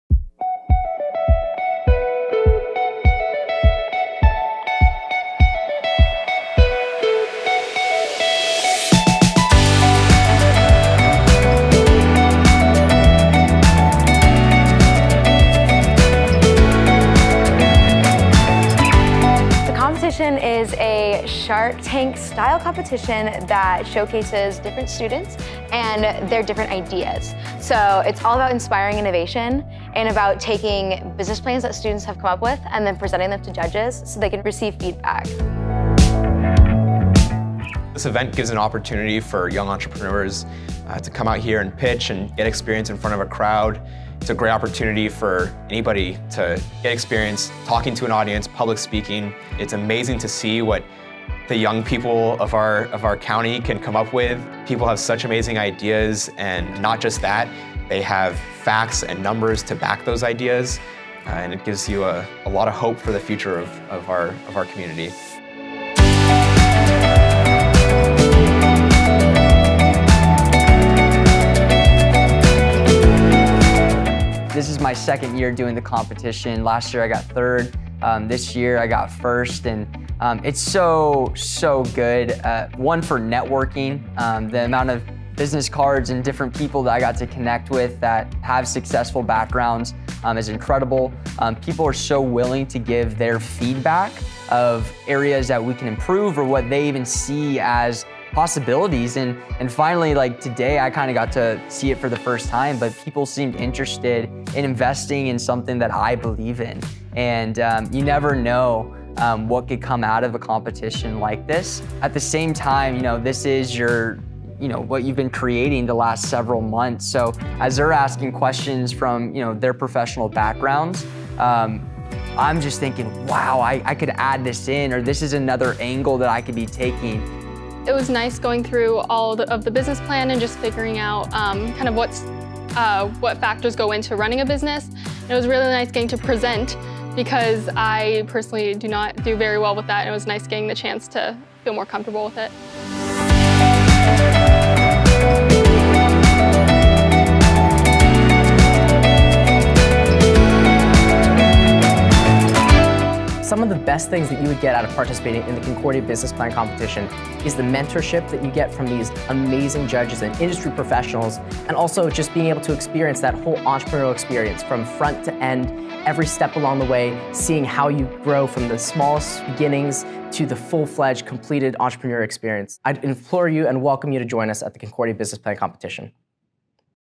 Concordia Business Plan Competition - 2022 Recap
Concordia University Irvine's annual Concordia Business Plan Competition (CBPC) includes two divisions: an undergraduate division composed of CUI undergraduate students and a high school division comprised of high school teams from the Orange County Area. See the recap of the 2022 Concordia Business Plan Competition, and hear from the participants, judges, and staff members who all participated in the success of this event.